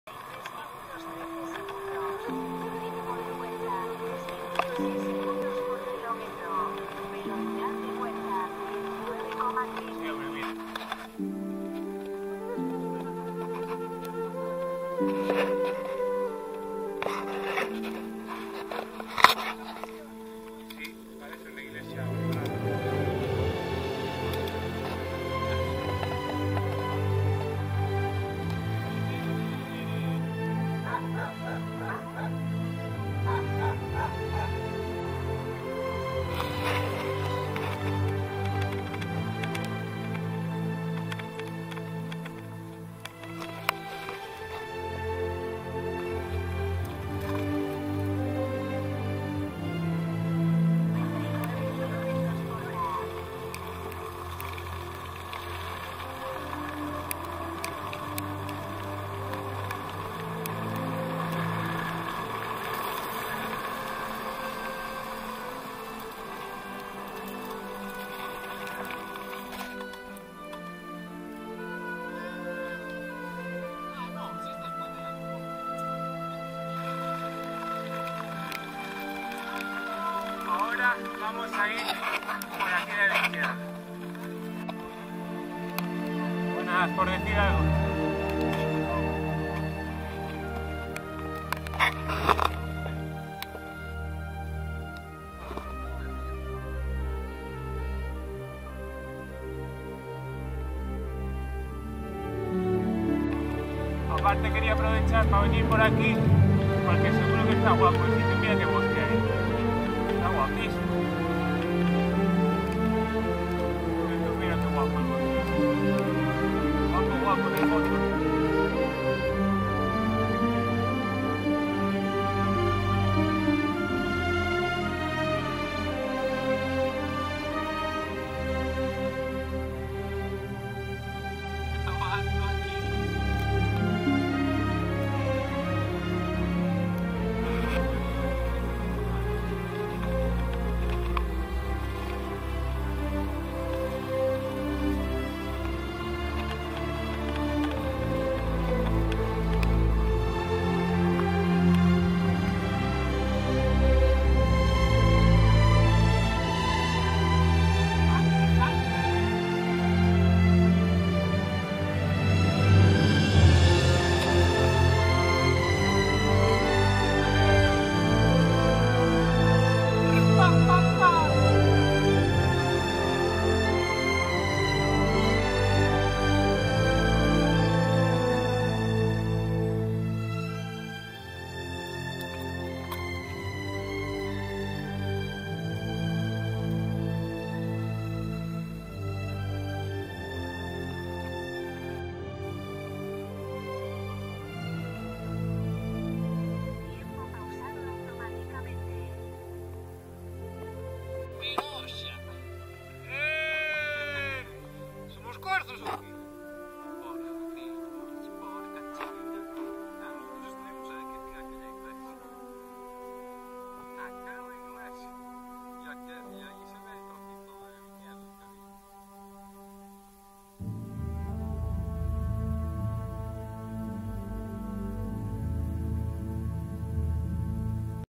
Subida Gulfariz y Castillo A Peroxa 28 06 2017
Subida al castillo de la Peroxa por Gulfariz, zona Barra de Miño